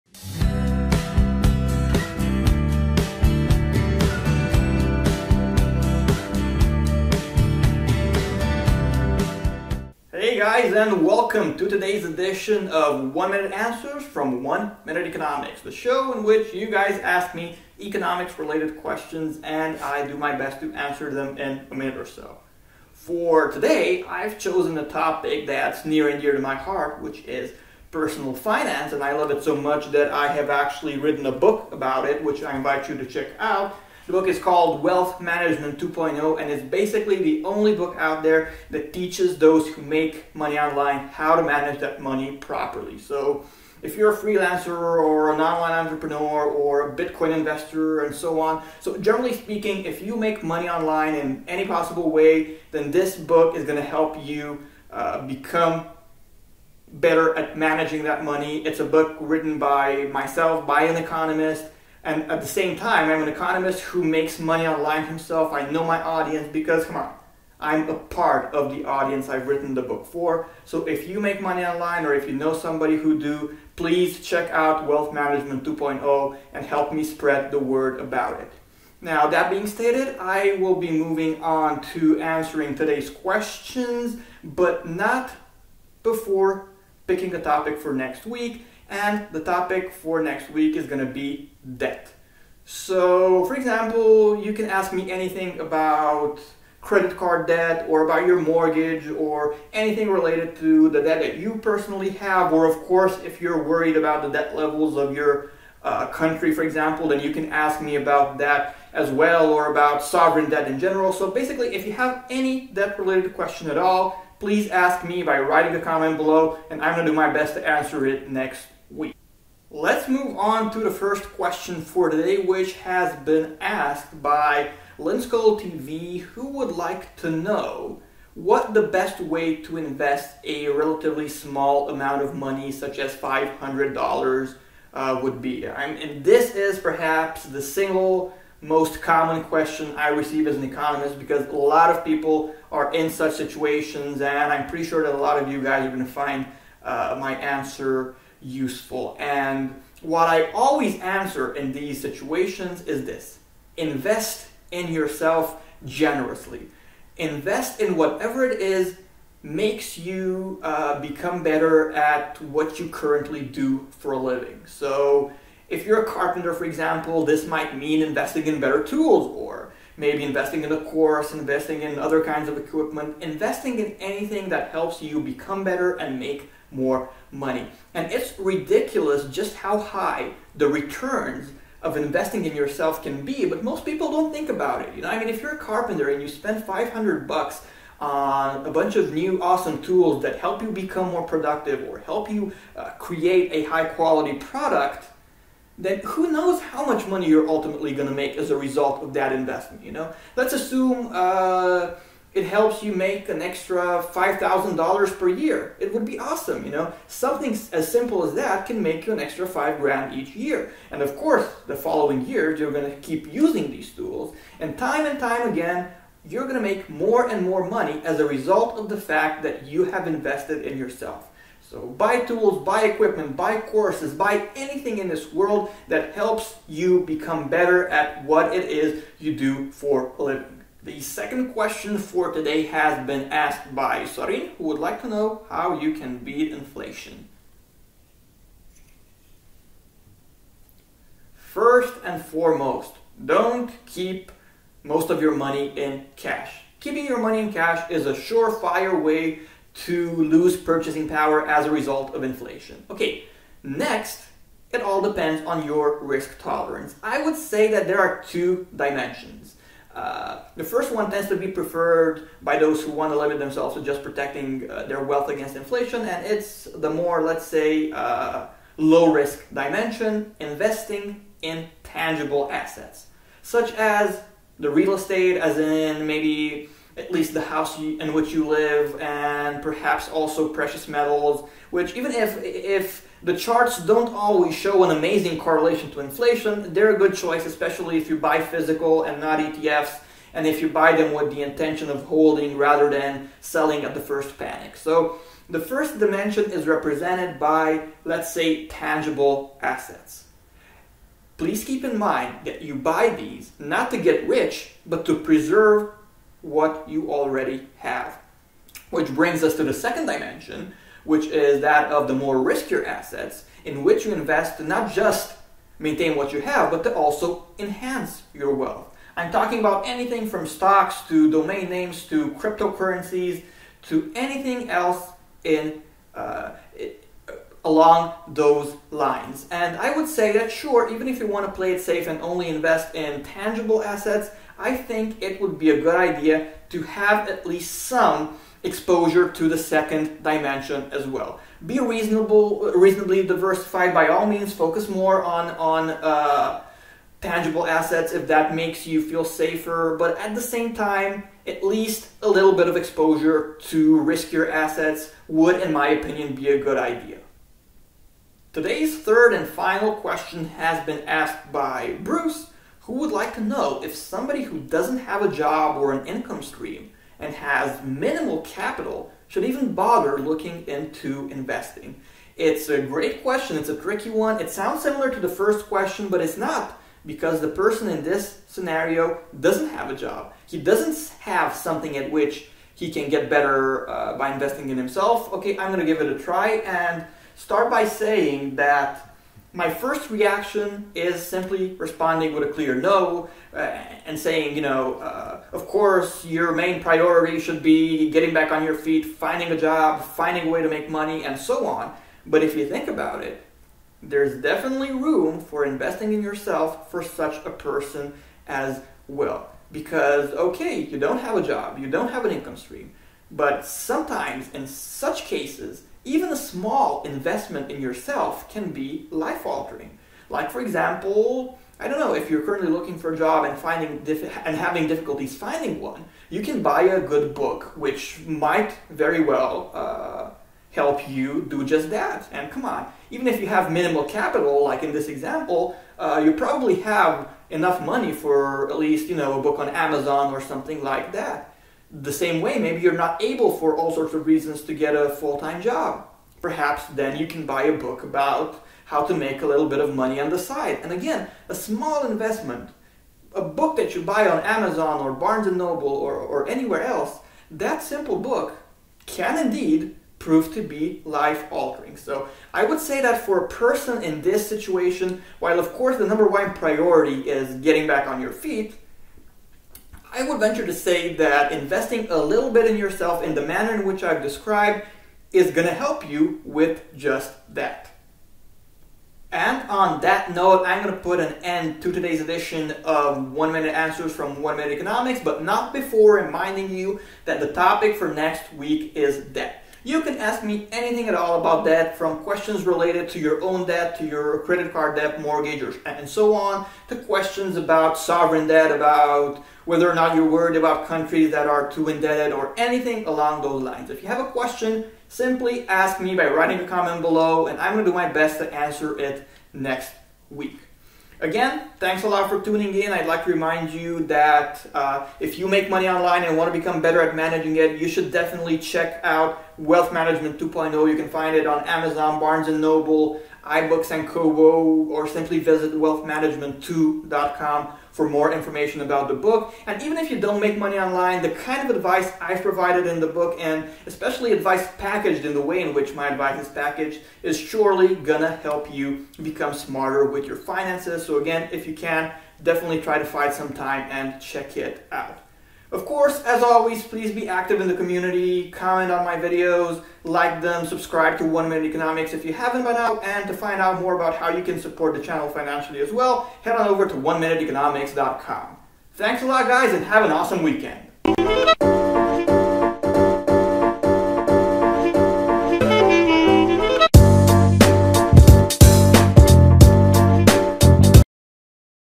How and Where to Invest Small Amounts (2 Q's) & Beating Inflation: One Minute Answers from OME
Three questions have been answered on this week's edition (this week's topic has been personal finance, whereas next week's topic will be debt) of One Minute Answers from One Minute Economics:

1) LINZZCOLD TV wanted to know how and where one should invest a small amount of money, for example $500 (click on the following link and you'll be taken directly to question #1

2) Sorin was interested in finding out how we can beat inflation, so I've shared my two cents about investing from the perspective of someone interested in beating inflation and have referred to some of the assets that tend to do well if beating inflation is your main goal (click on the following link and you'll be taken directly to question #2

3) Bruce wanted to find out if investing is worth it for someone without a job/income source and minimal capital. Let's find out how and where should such a person invest... if he should invest at all, that is (click on the following link and you'll be taken directly to question #3